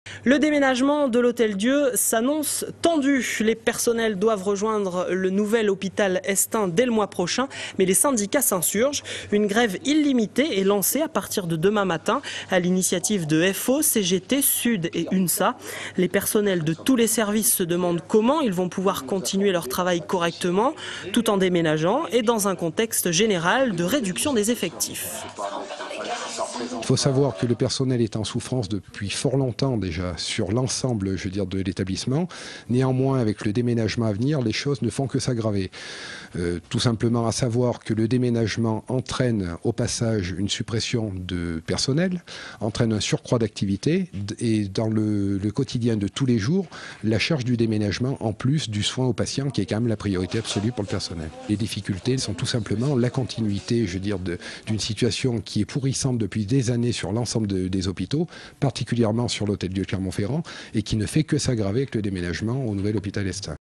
Hôtel Dieu, Un déménagement tendu (Clermont)
L'Hôpital de L'Hotel Dieu déménage. Le personnel soignant se demande encore comment ils vont pouvoir exercer correctement leur travail dans de telles circonstances. (DROITS RESERVES . Pour toute exploitation sur TV, web, mobile, aller